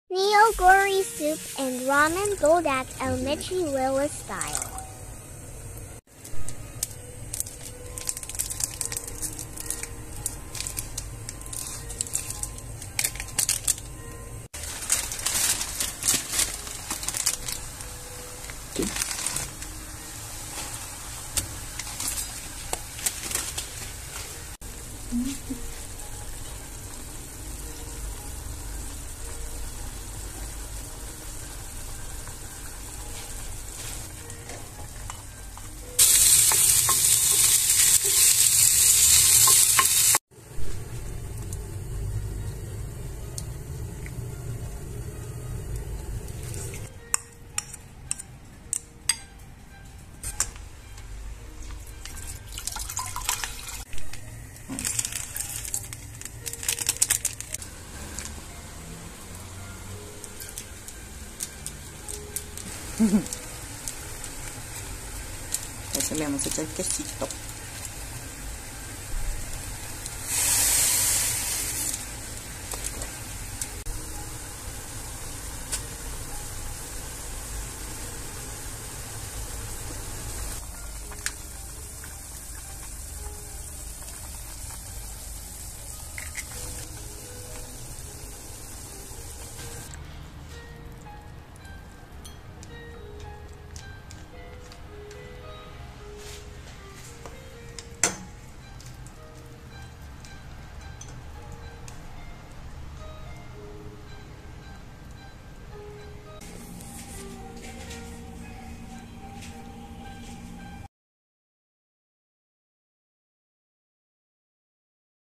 ¿cómo preparar Ramen Buldak y Neoguri? Al MichiLilith Style 😻
Nuestra particular forma de preparar el ramen buldak y neoguri para que lo disfrutes, si no sabes dónde adquirir esos y otros productos de kfood visita nuestro Instagram @michililith y conoce los